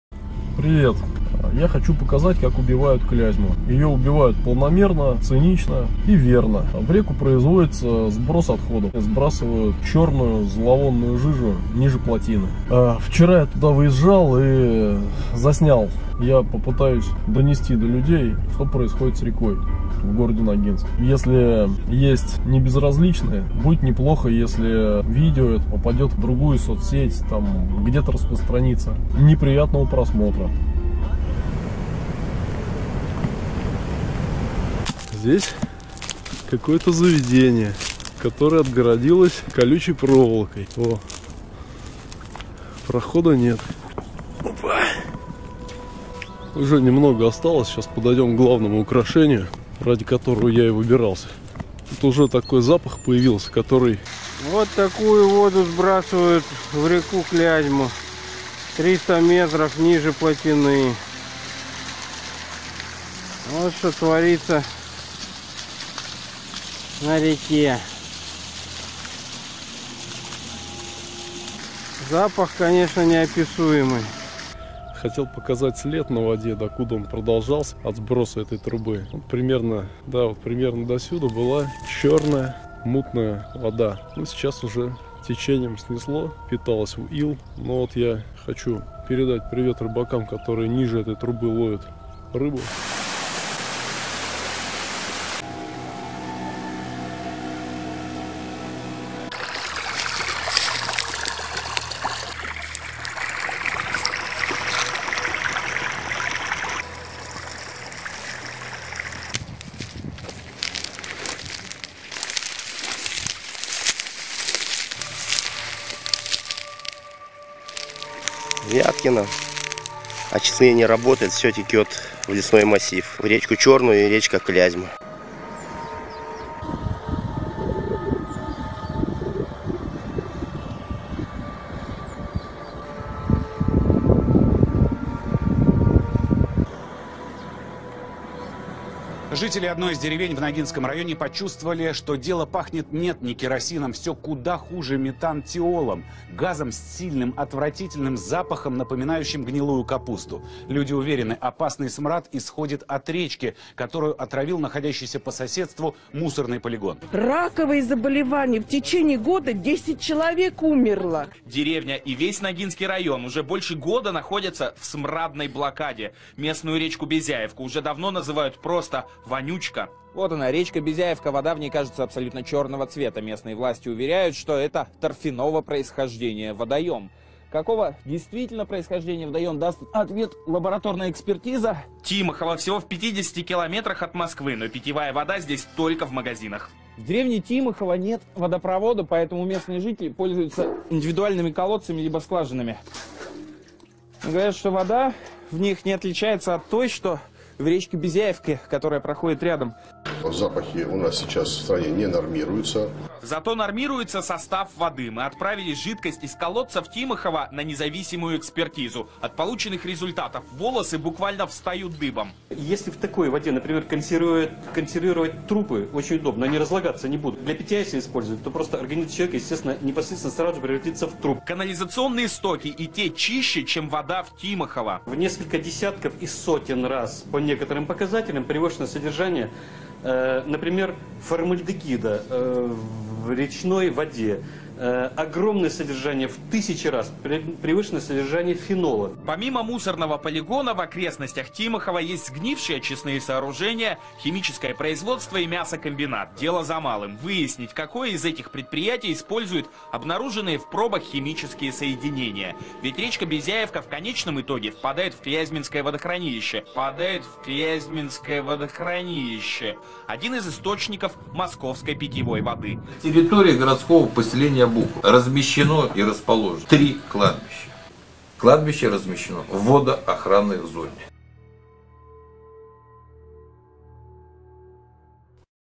Сброс отходов в реку Клязьма, г. Ногинск, г. Обухово, как соседи травят друг друга.
Как живут люди в городах которые находятся на берегах одной реки?
Накакай на нижнего соседа, это так называется...
Экспертиза показывает, вода опасна для жизни.